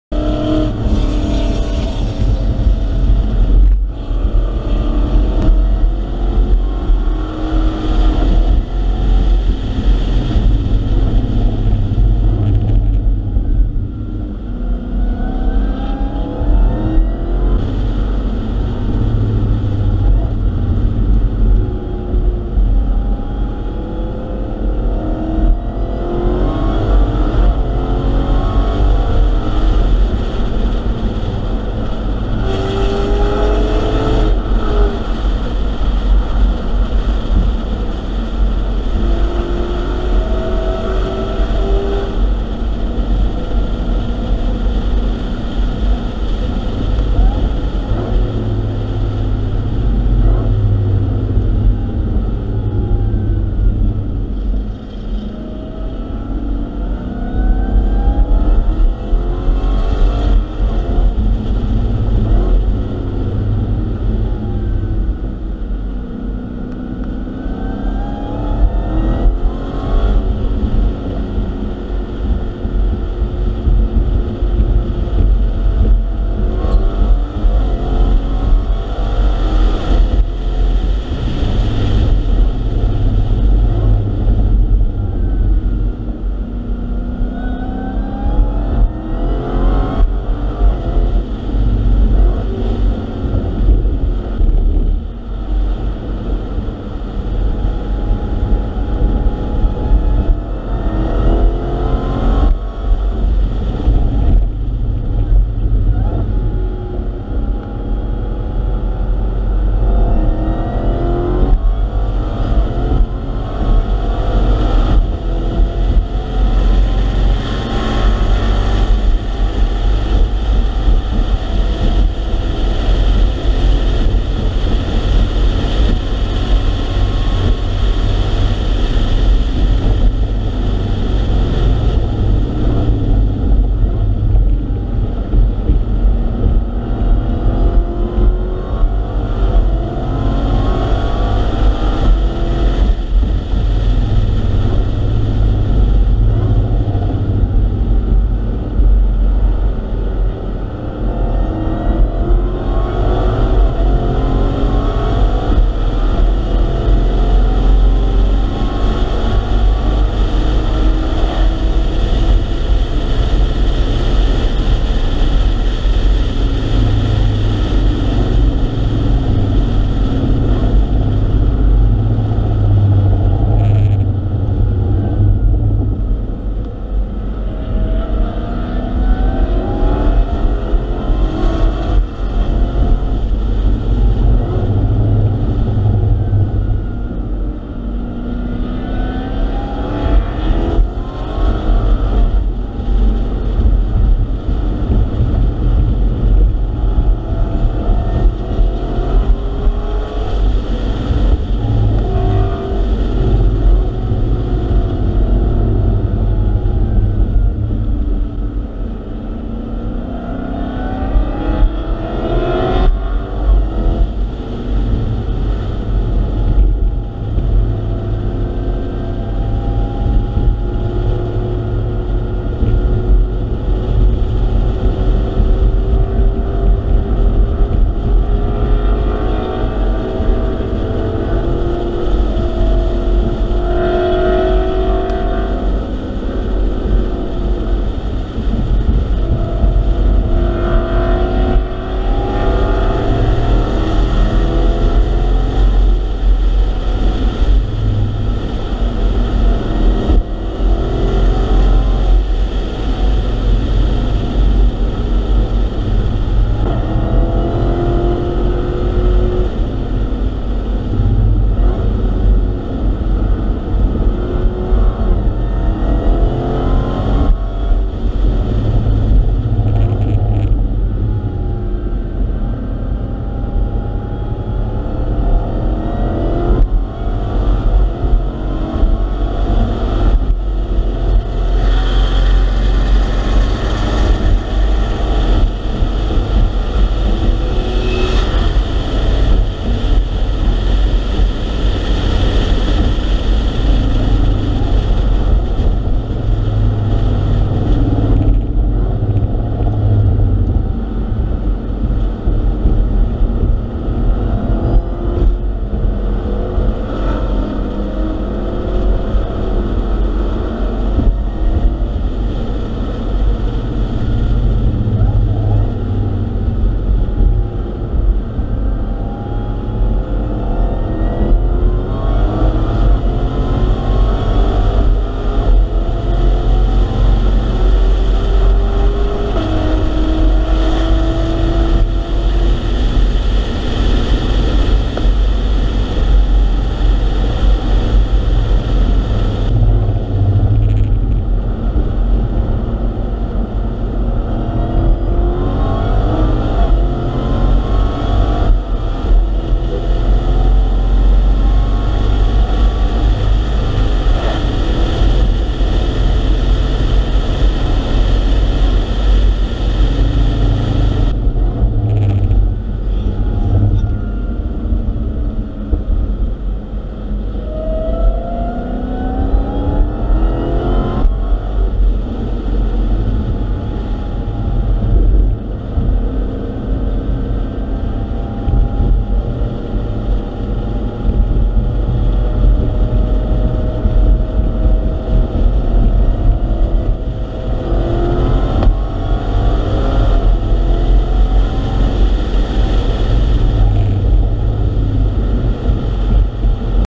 Point Byes for the Redeye
Session #2 RA MVP Track Time Saturday May 22.
More entertaining than just watching me lap after lap.  Look at what we pass and/or put up with on the Road Course in a session.  BUT, it is a BLAST!  Enjoy the fun captured.